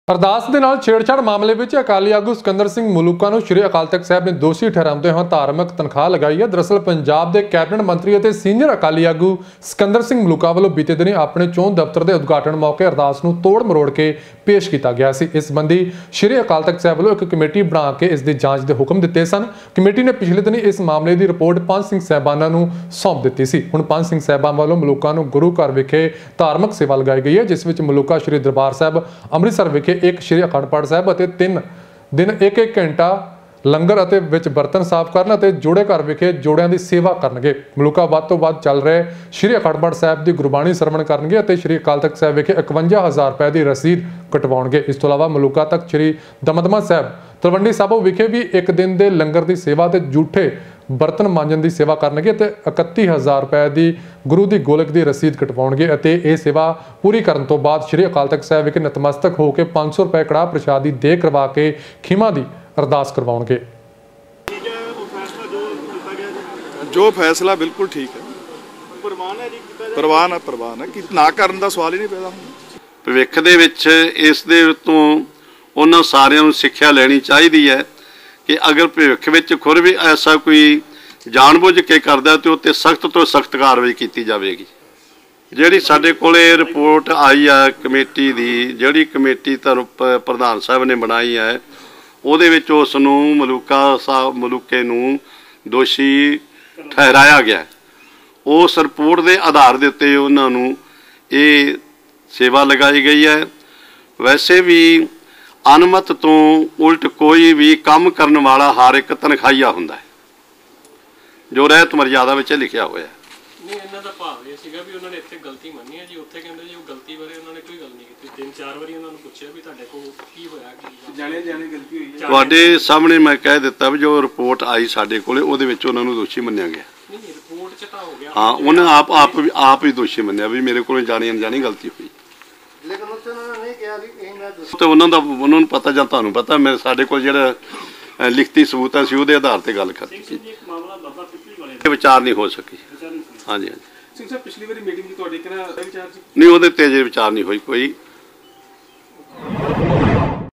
Ardas 'imitation' row Maluka awarded religious punishment
ਅਰਦਾਸ ਮਾਮਲਾ: ਸਿੰਕਦਰ ਸਿੰਘ ਮਲੂਕਾ ਨੂੰ ਧਾਰਮਿਕ ਸਜ਼ਾ